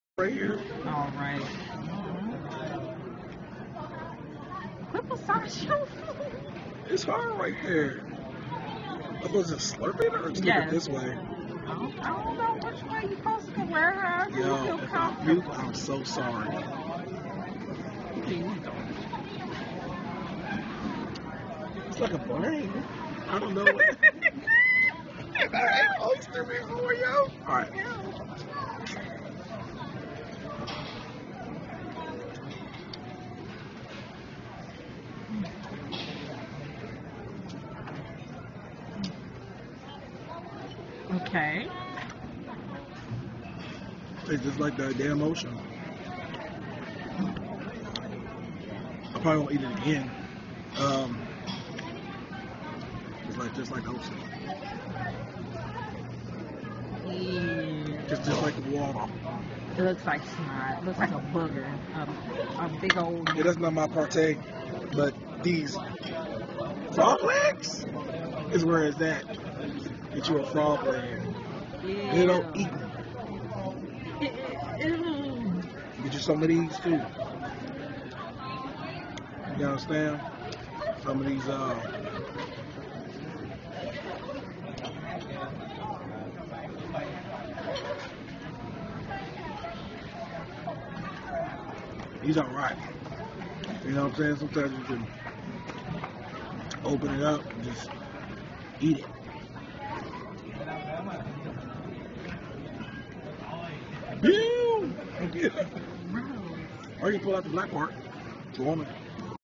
At Hollywood Casino Buffet
Alex tasting an oyster for the first time